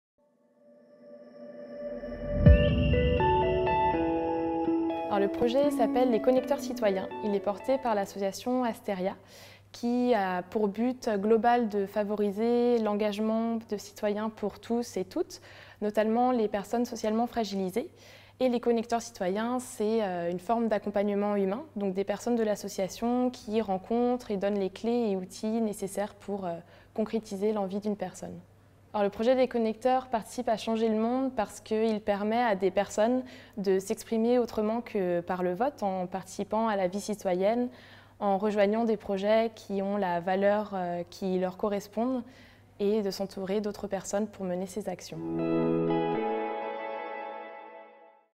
Les connecteurs citoyens, qu'est-ce que c'est ?
🌻 Astérya est lauréat de l'appel à projet de la Fondation des Petits frères des Pauvres grâce à son pôle les Connecteurs Citoyens pour son projet destiné à cultiver et promouvoir les liens intergénérationnels. Mais au fait, les connecteurs citoyens, qu'est-ce que c'est ? 🤔

🎙 I Cécile, connectrice citoyenne à Astérya I
📍 I Remise de prix à la Belleviloise I
📆 I 9 avril 2018 I